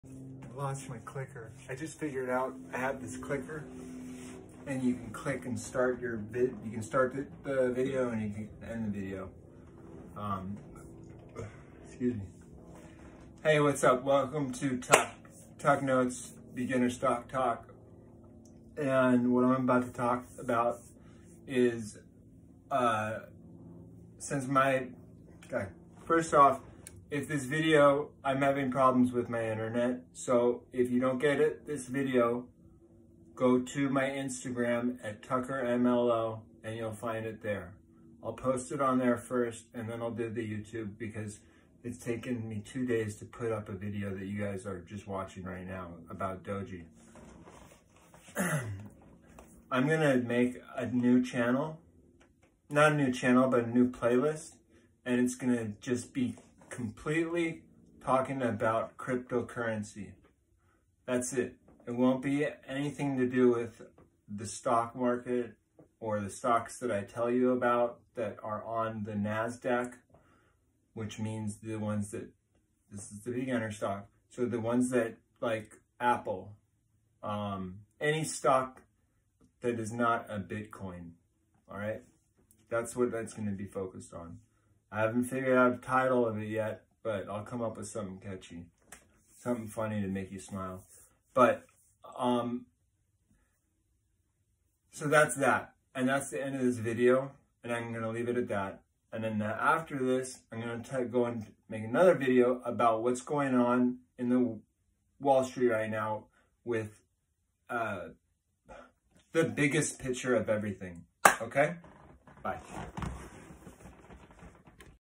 HOW TO MAKE A NEW PLAYLIST ON YOUTUBE ‘Tuck’s Crypt Currency Coin Talk’ HOW TO WATCH IG
In this video I go over HOW TO MAKE A NEW PLAYLIST ON YOUTUBE ‘Tuck’s Crypt Currency Coin Talk’ HOW TO WATCH IG @tuckermlo